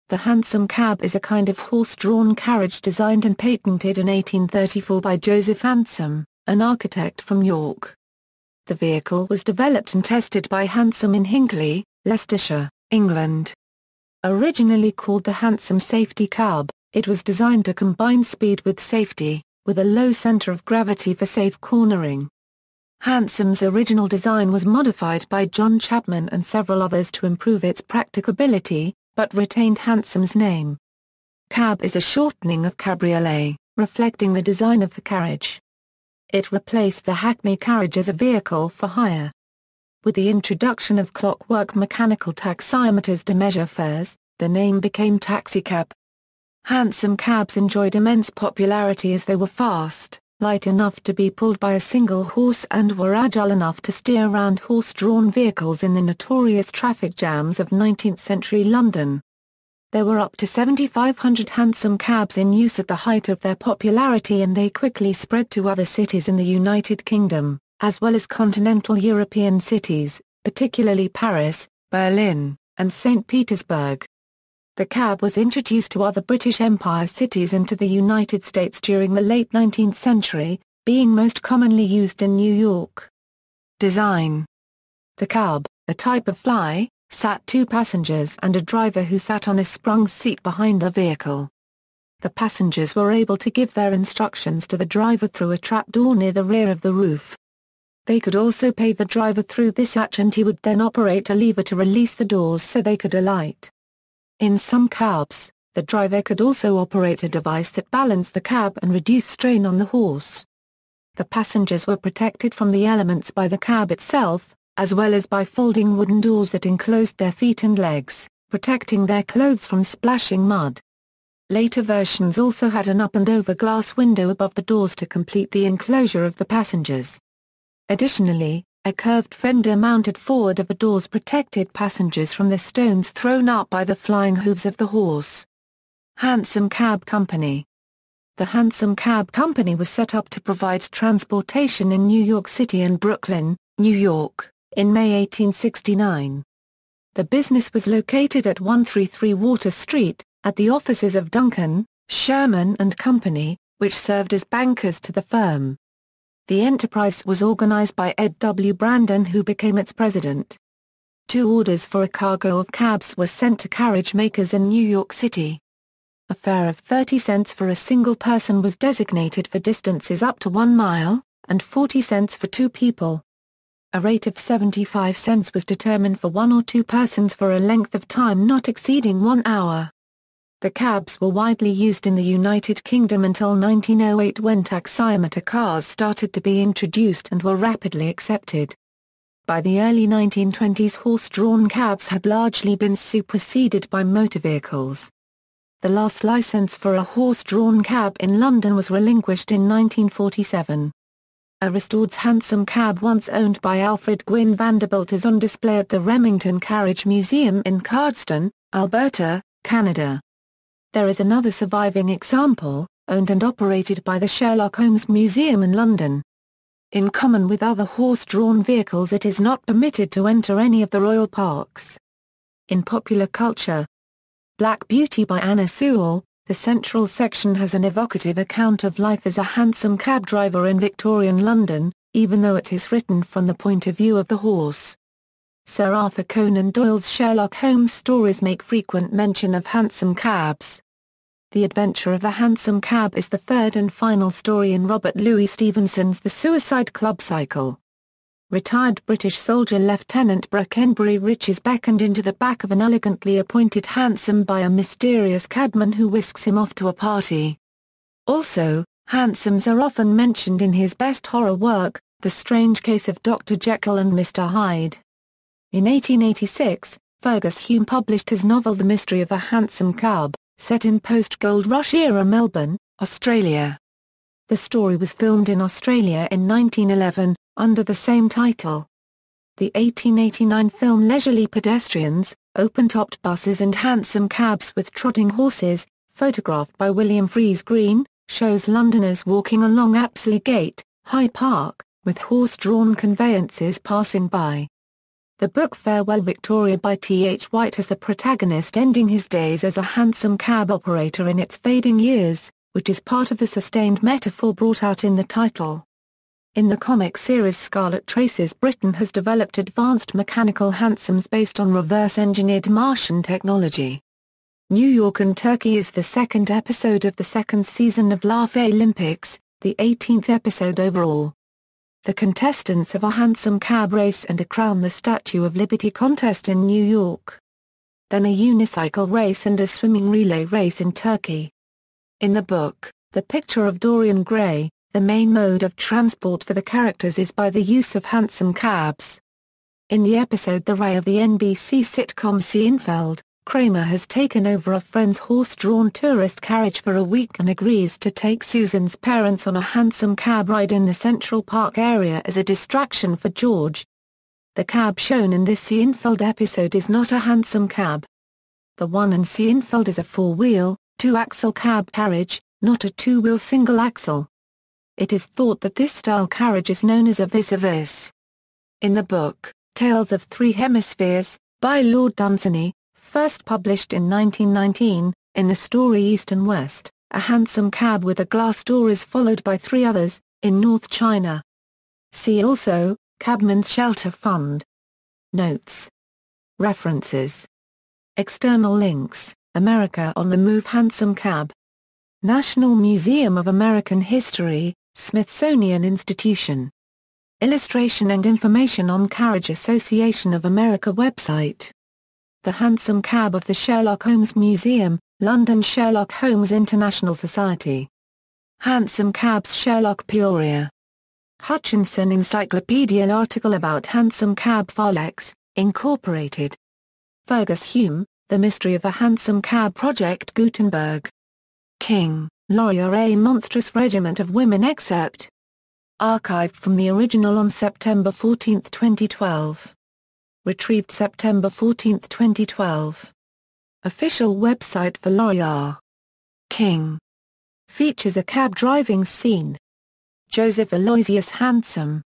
Hansom cab
The hansom cab is a kind of horse-drawn carriage designed and patented in 1834 by Joseph Hansom, an architect from York. The vehicle was developed and tested by Hansom in Hinckley, Leicestershire, England. Originally called the Hansom safety cab, it was designed to combine speed with safety, with a low centre of gravity for safe cornering. Hansom's original design was modified by John Chapman and several others to improve its practicability, but retained Hansom's name.
Cab is a shortening of cabriolet, reflecting the design of the carriage. It replaced the hackney carriage as a vehicle for hire; with the introduction of clockwork mechanical taximeters to measure fares, the name became taxicab.